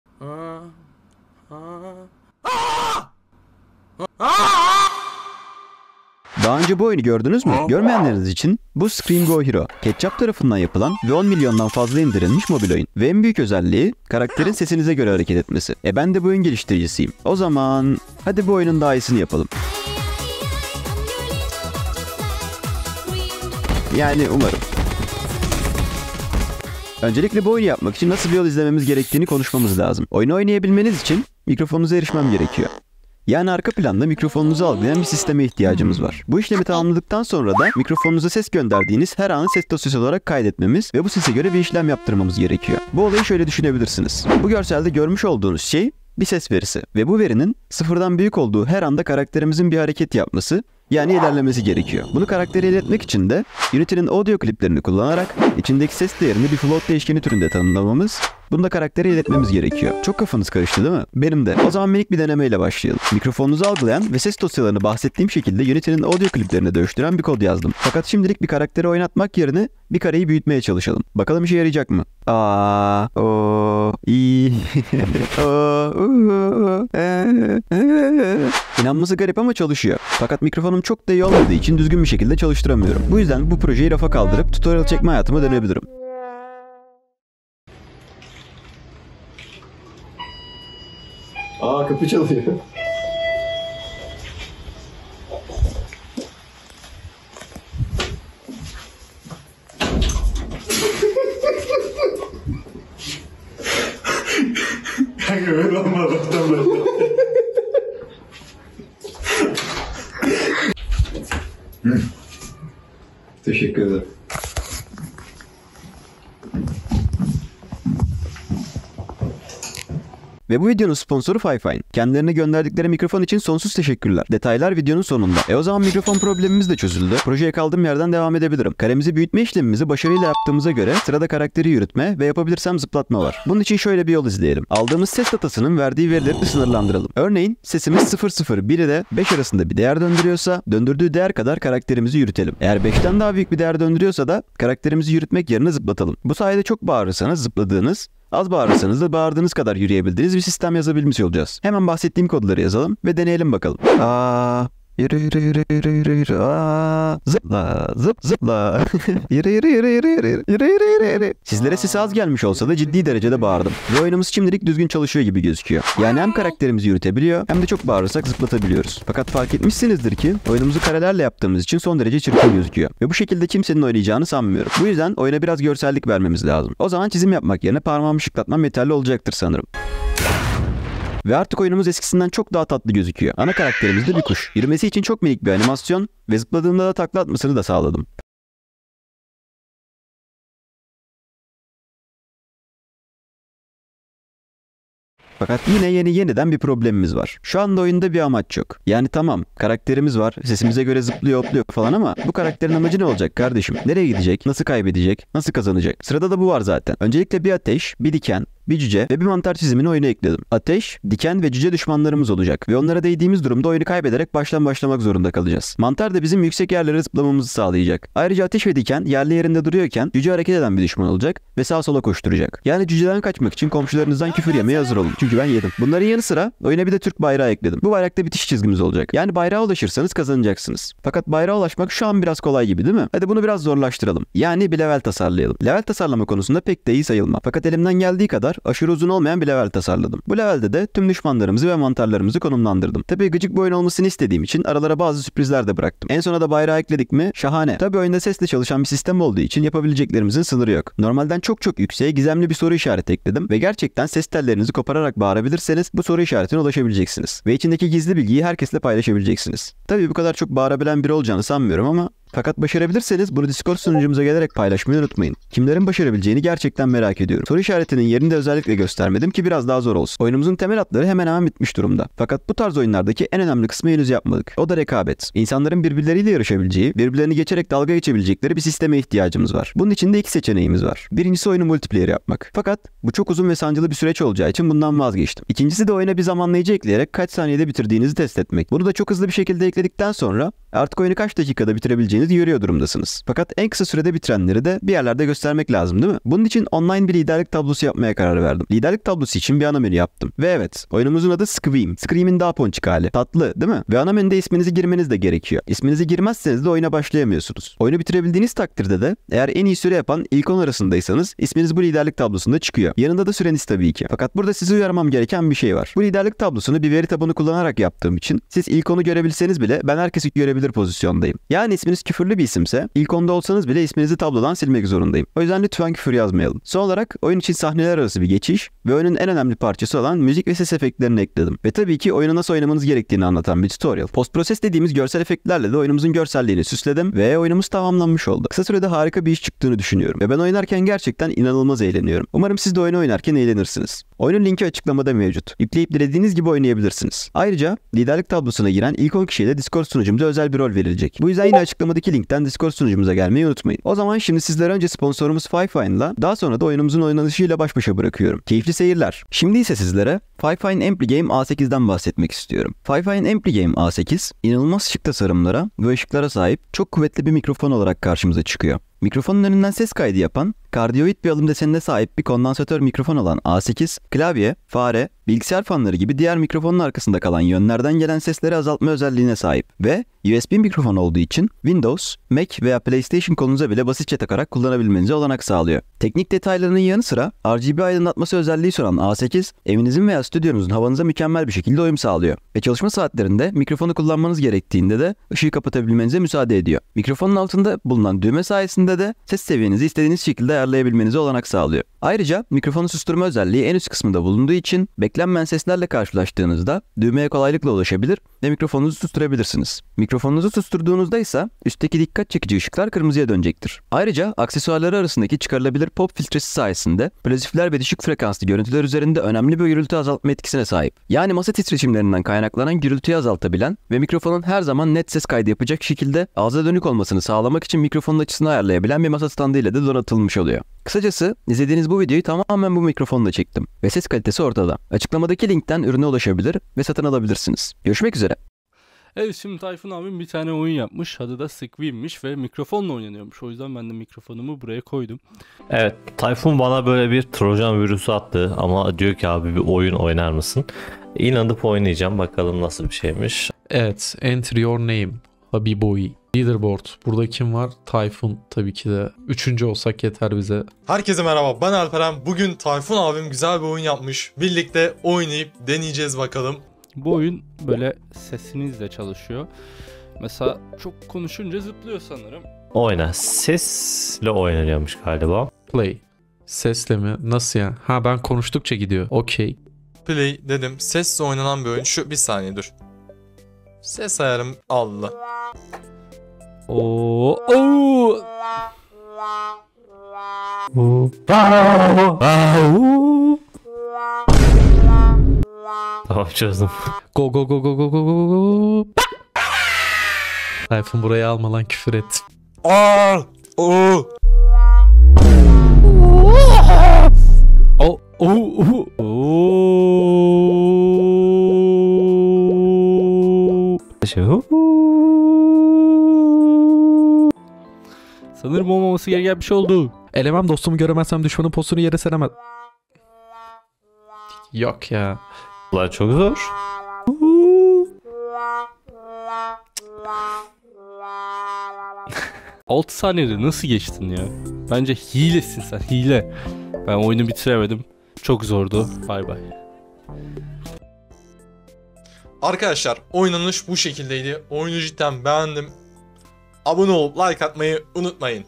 Bu Oyunu Oynamak için BAĞIRMAK ZORUNDASIN! | FIFINE Ampligame A8
Ketchapp'ın ünlü oyunu "Scream Go Hero"yu örnek alarak yaptığım ve sesinizle kontrol edebildiğiniz oyunu eğlenceli bir devlog halinde sizlere sunmaya çalıştım. Ve bu oyunu oynarken tek ihtiyacınız olan şey sesiniz. Umarım beğenirsiniz, iyi seyirler!

Oyunu oynadıkları ve tepki verdikleri için Marcus7z, Habiboi ve ka6an'a çok teşekkürler!!

Zamanlayıcılar
00:00 Intro
00:26 Devlog
05:59 Sponsor: FIFINE
07:18 Tepkiler: Marcus7z, ka6an, Habiboi